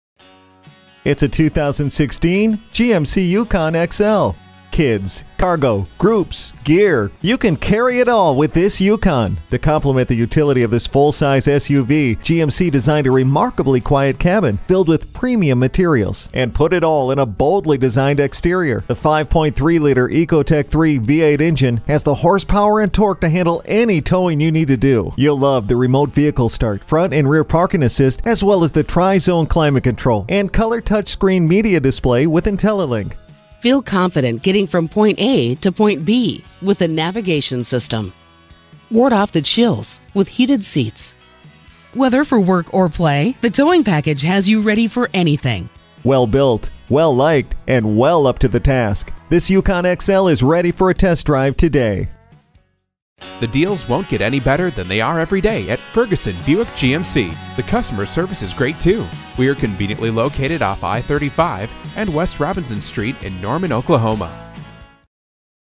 New 2016 GMC YUKON XL C1500 SLT Norman OK Oklahoma-City, OK G9363 - SOLD
Year: 2016
Make: GMC
Model: YUKON XL C1500 SLT
Engine: 5.3 Liter 8 Cylinder
Transmission: Automatic
Color: G1E CRIMSON RED TINTCOAT
Interior: H2Y COCOA DUNE
Stock #: G9363
VIN: 1GKS1GKC2GR266393

Address:
1015 North Interstate Drive